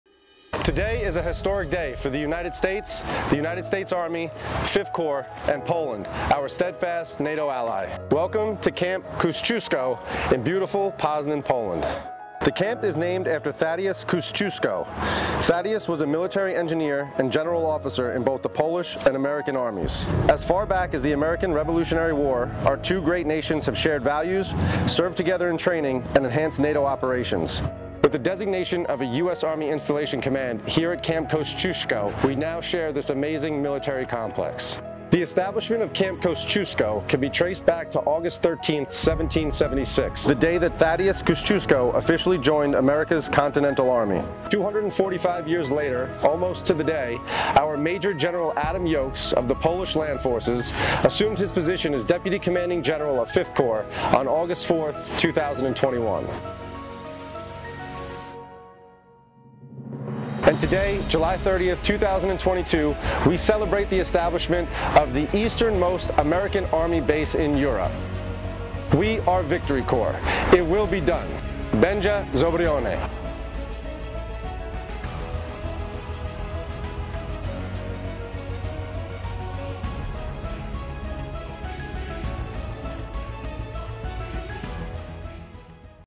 Camp Kosciuszko
The U.S. Army Garrison Poland supports the V Corps mission by maintaining facilities at Camp Kosciuszko in the city of Poznan, Poland.

Camp Kosciuszko is named after Tadeusz (Thaddeus) Kosciuszko, an accomplished engineer and American revolutionary war hero.  In 1776, Kościuszko moved to North America, where he took part in the American Revolutionary War as a colonel in the Continental Army. As an engineer, he designed and oversaw the construction of state-of-the-art fortifications, including those at West Point.  In 1783, in recognition of his services, the Continental Congress promoted him to brigadier general.  Upon returning to Poland in 1784, Kościuszko was commissioned as a major general in the Polish–Lithuanian Commonwealth Army.